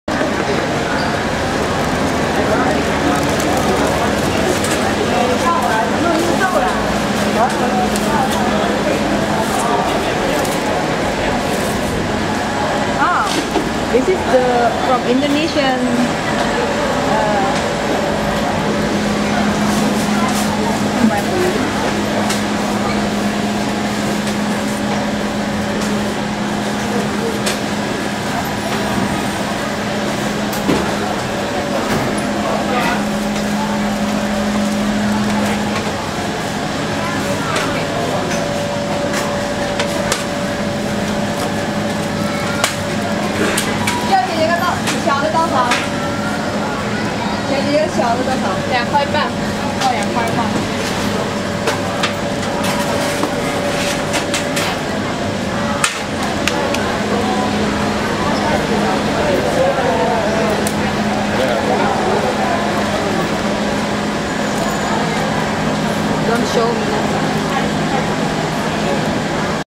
Singaporean bread
large selection of cakes and breads available in Singapore's Chinatown.